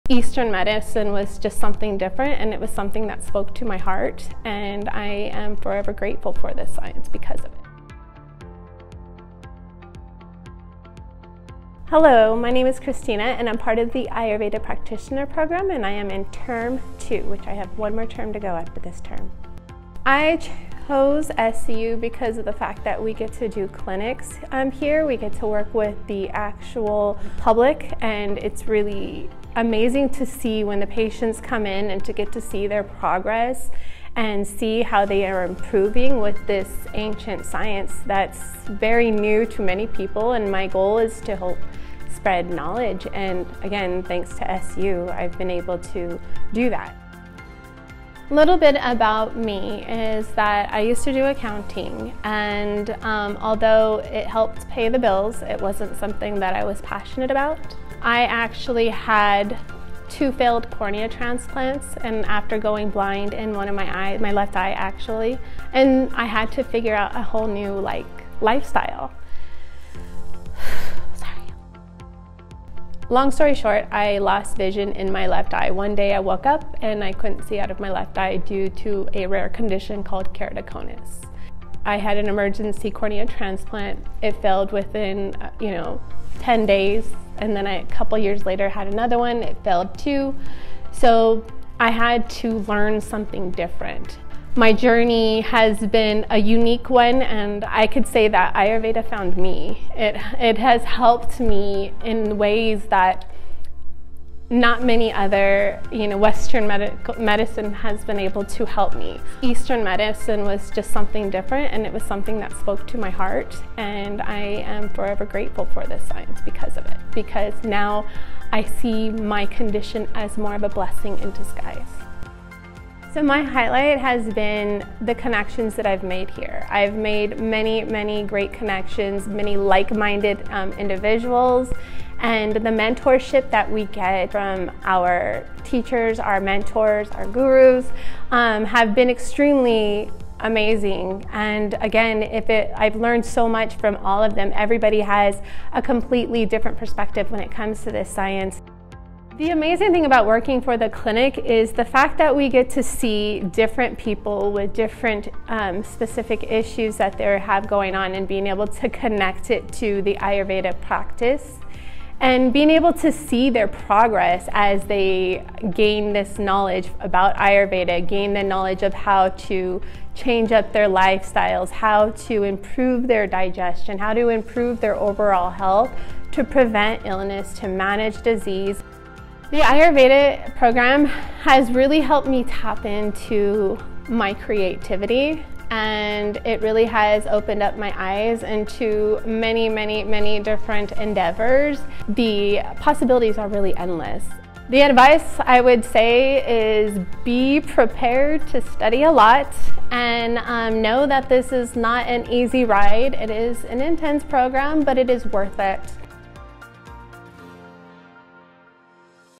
How Ayurveda Change My Life
Introducing Christina! Currently enrolled in our Ayurveda Practitioner program at SCU, she seizes every opportunity to pursue her passion. Discover how Ayurveda supports her in managing Keratoconus, providing holistic healing and relief. Christina's journey is a testament to the transformative power of natural medicine!